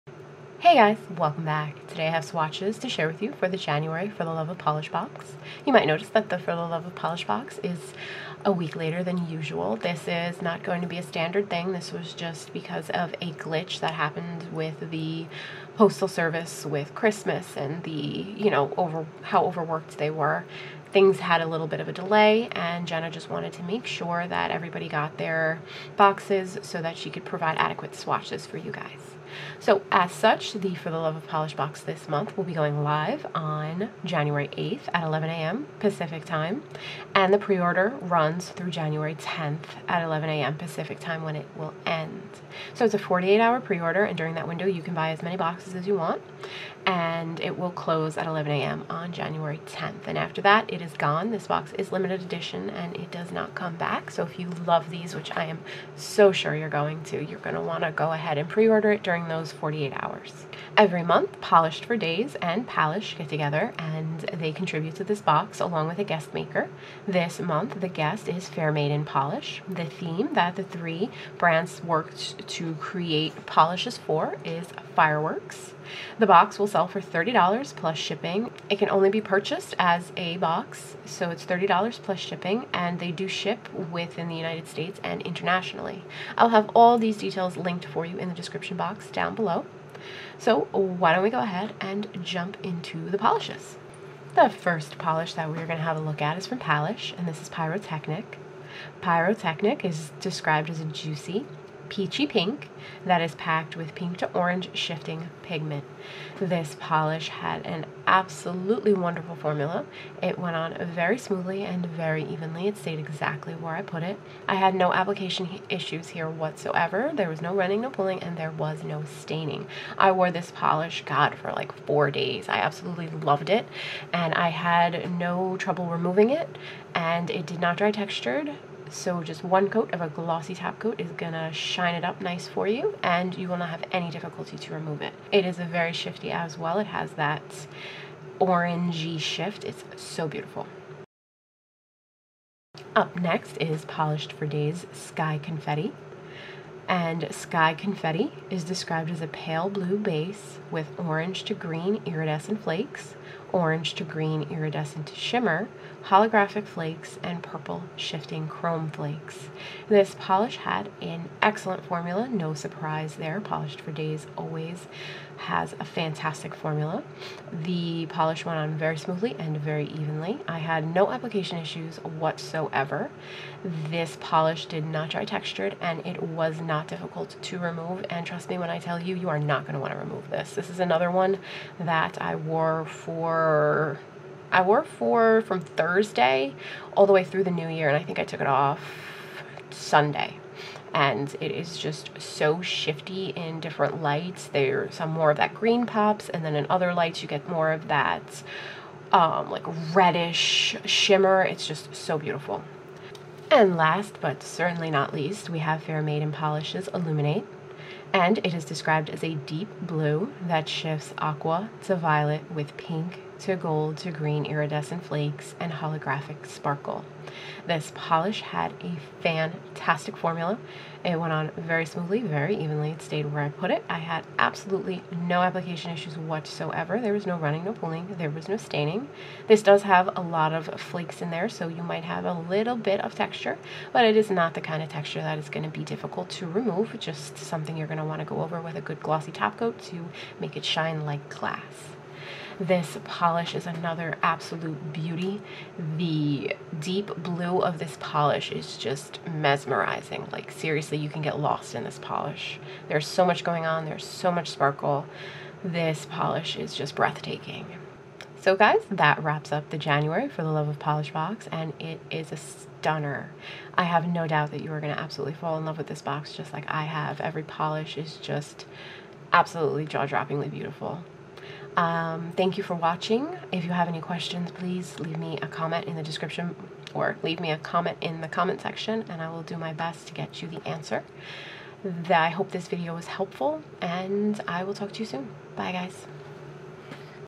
Swatch & Review | For The Love Of Polish - January 2021
Items sent for photographs and an honest review.
All descriptions are provided by the brand, but opinions are my own.

Guest: Fair Maiden Polish
Theme: Fireworks

Preorder: 48 hr preorder from 1/8/21 - 1/10/21 11am PST.
Price: $30.00 plus shipping.
Shipping: USA and International.

Polish In Order Of Appearance:
Pahlish ”Pyrotechnic”
Polished For Days “Sky Confetti”
Fair Maiden Polish “Illuminate”